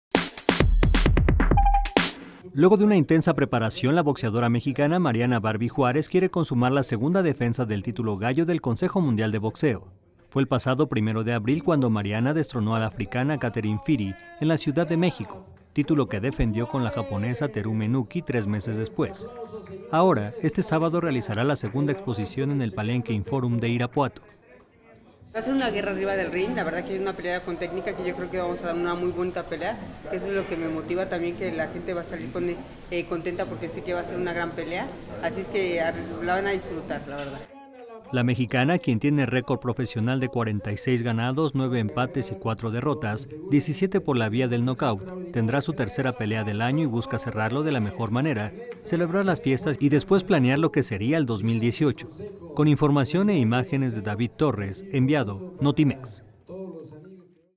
Mariana “Barby” Juárez expondrá título gallo del CMB
La "Barby", quien pesó 53.500 kilogramos, el límite de la división y 500 gramos más que su rival, reconoció la calidad de Graf, segura de que ofrecerán una “guerra” en el ring.